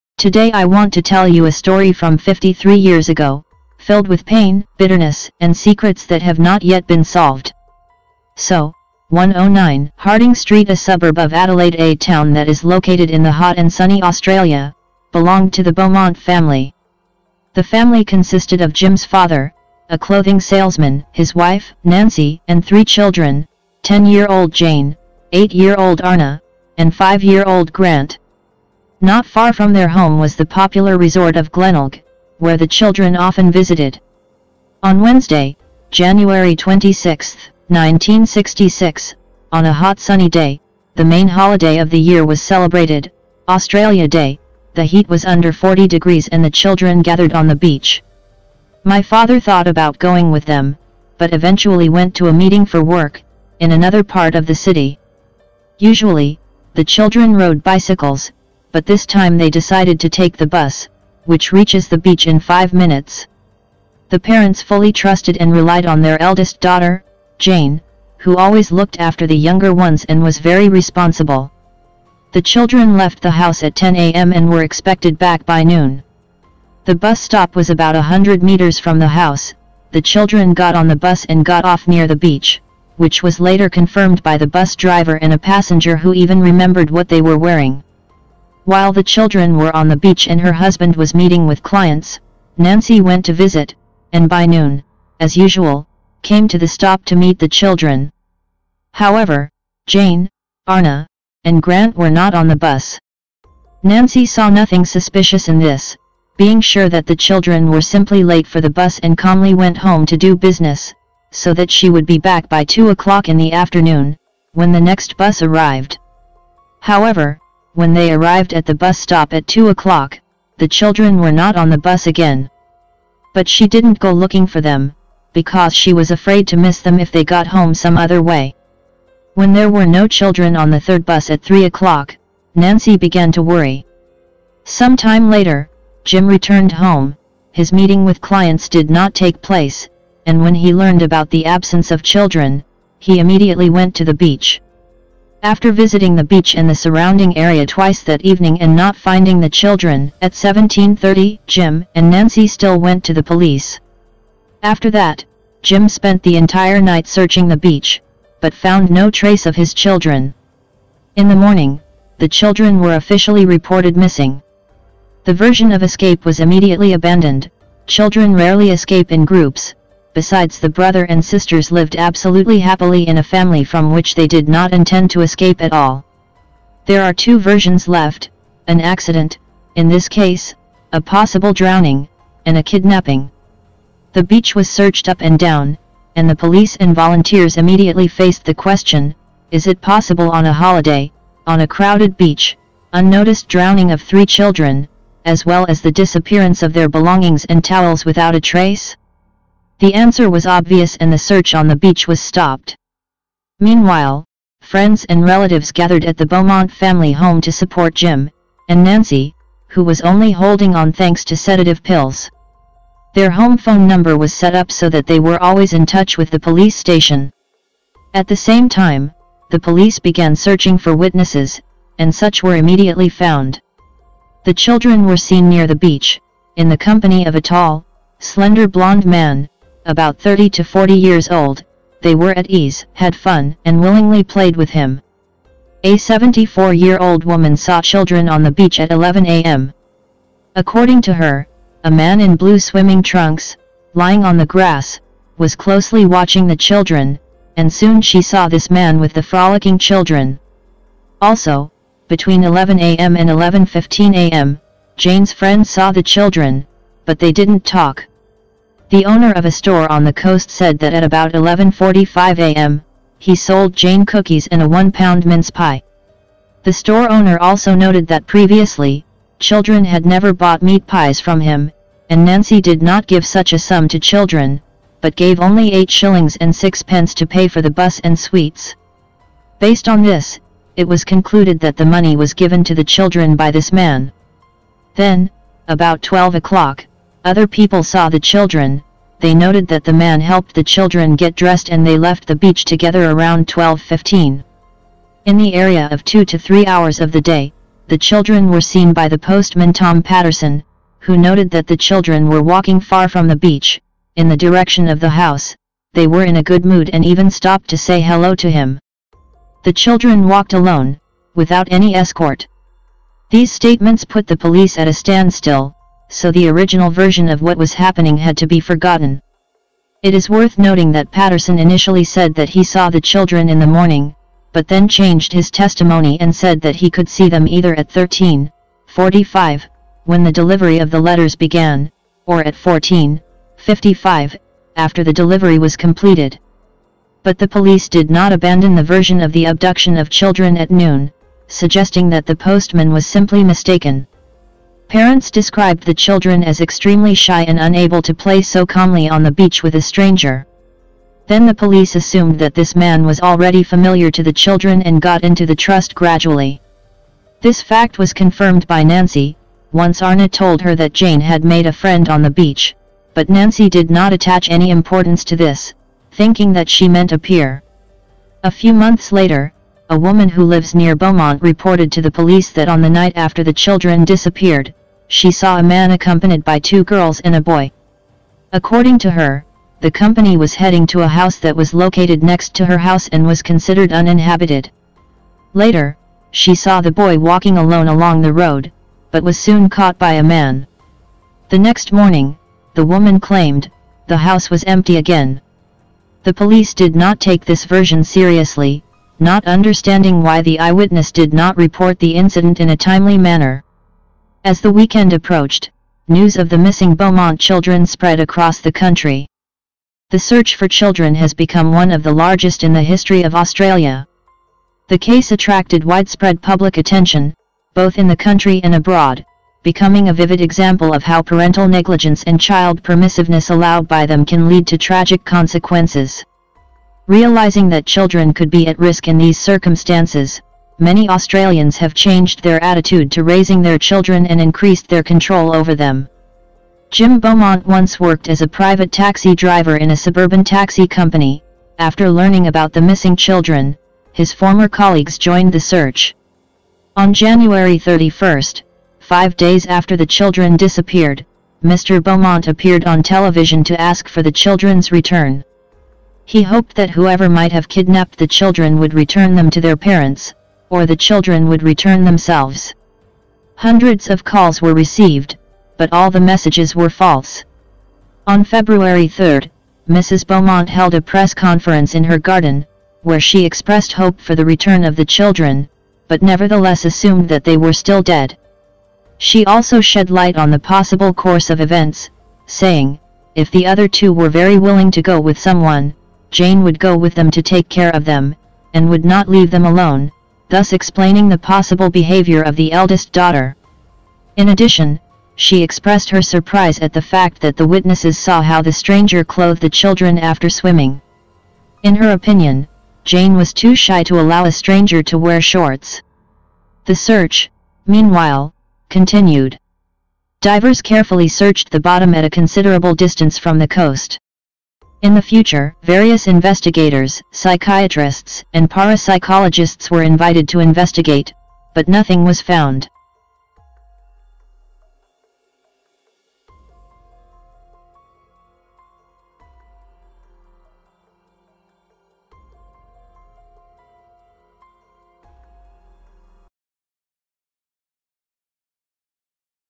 The disappearance of the BEAUMONT CHILDREN What really happened True Story
Today I want to tell you a story from 53 years ago, filled with pain, bitterness, and secrets that have not yet been solved.

So, 109 Harding street, a suburb of Adelaide-a town that is located in the hot and Sunny Australia, belonged to the beaumont family. The family consisted of Jim's father, a clothing salesman, his wife, Nancy, and three children, 10-year-old Jane, 8-year-old Arnna, and 5-year-old Grant. Not far from their home was the popular resort of Glenelg, where the children often visited.

On Wednesday, January 26, 1966, on a hot Sunny day, the main holiday of the year was celebrated - Australia day, the heat was under 40° and the children gathered on the beach. My father thought about going with them, but eventually went to a meeting for work, in another part of the city. Usually, the children rode bicycles, but this time they decided to take the bus, which reaches the beach in 5 minutes. The parents fully trusted and relied on their eldest daughter, Jane, who always looked after the younger ones and was very responsible. The children left the house at 10 a.m. and were expected back by noon. The bus stop was about a hundred meters from the house, the children got on the bus and got off near the beach, which was later confirmed by the bus driver and a passenger who even remembered what they were wearing.

While the children were on the beach and her husband was meeting with clients, Nancy went to visit, and by noon, as usual, came to the stop to meet the children. However, Jane, Arnna, and Grant were not on the bus. Nancy saw nothing suspicious in this, being sure that the children were simply late for the bus and calmly went home to do business, so that she would be back by two o'clock in the afternoon, when the next bus arrived. However, when they arrived at the bus stop at two o'clock, the children were not on the bus again. But she didn't go looking for them, because she was afraid to miss them if they got home some other way. When there were no children on the third bus at 3 o'clock, Nancy began to worry. Some time later, Jim returned home, his meeting with clients did not take place, and when he learned about the absence of children, he immediately went to the beach. After visiting the beach and the surrounding area twice that evening and not finding the children, at 17:30, Jim and Nancy still went to the police. After that, Jim spent the entire night searching the beach, but found no trace of his children. In the morning, the children were officially reported missing. The version of escape was immediately abandoned, children rarely escape in groups, besides the brother and sisters lived absolutely happily in a family from which they did not intend to escape at all. There are two versions left - an accident (in this case, a possible drowning) and a kidnapping.